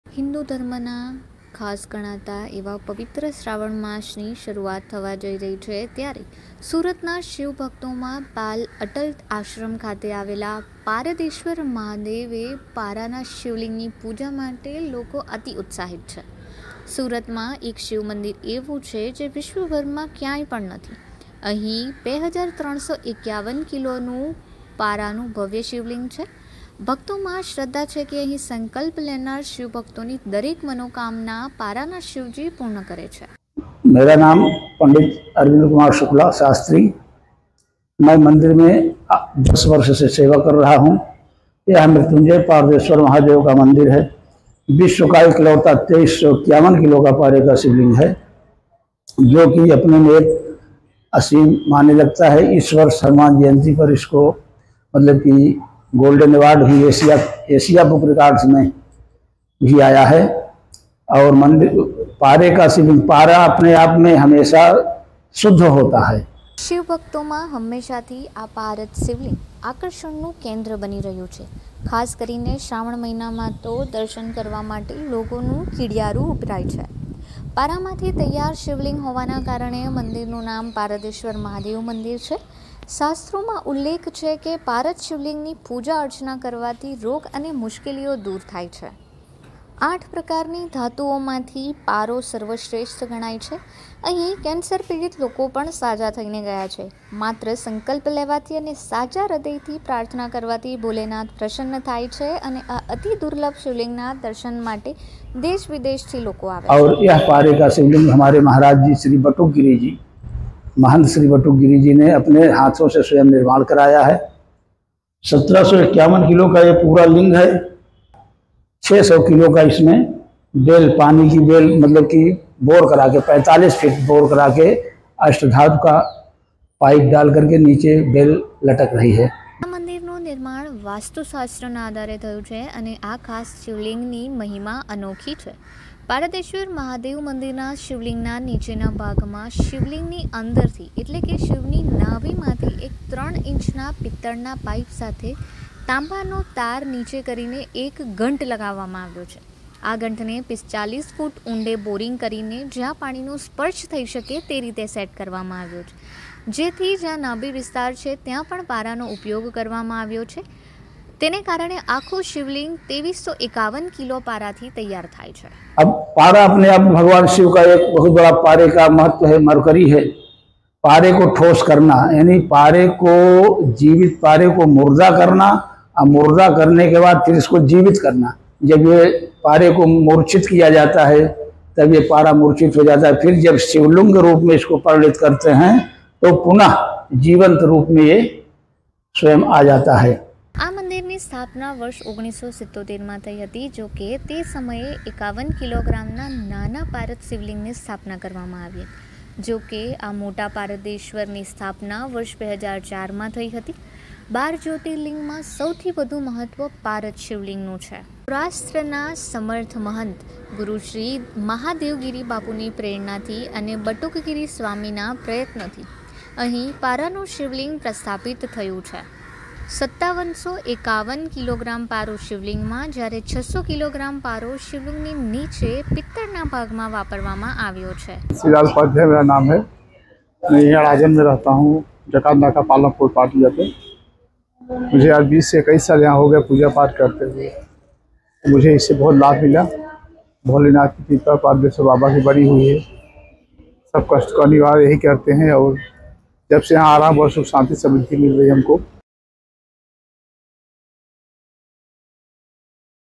Surat News | વાસ્તુશાસ્ત્રના આધારે થયું છે આ મંદિરનું નિર્માણ | Mahadev Temple
Among the Shiva devotees of Surat, people are excited to worship the Shivlinga of Paradeshwar Mahadev Para at Pal Atal Ashram. There is a Shiva temple in Surat which is not found anywhere in the world. There is a magnificent Shivlinga of 2,351 kg of mercury. Devotees believe that every wish of the Shiva devotees who take a vow here is fulfilled by Shivaji of mercury.

સુરતના શિવભક્તોમાં પાલ અટલ આશ્રમ ખાતે આવેલા પારદેશ્વર મહાદેવે પારાના શિવલિંગની પૂજા માટે લોકો ઉત્સાહિત છે. સુરતમાં એક શિવ મંદિર એવું છે જે વિશ્વભરમાં ક્યાંય પણ નથી અહીં 2,351 કિલો પારાનું ભવ્ય શિવલિંગ છે.ભક્તોમાં શ્રદ્ધા છે કે અહીં સંકલ્પ લેનાર શિવભક્તોની દરેક મનોકામના પારાના શિવજી પૂર્ણ કરે છે.